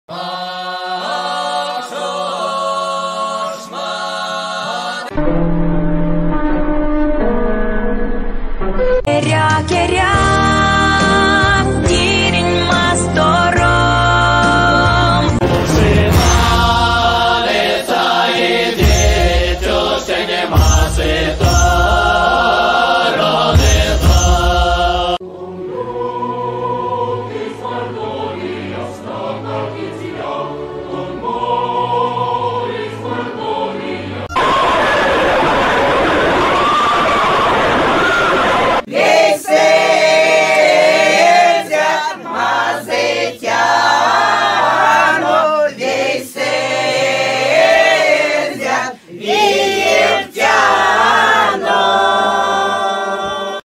POV: you live in Erzyan Mastor in...
Erzyan Mastor is the country of the indigenous Erzya people, who live in the Middle Volga region (Eastern Europe).

I present to you a brief history of Erzyan Mastor.

P.S. Sorry for grammatical mistakes.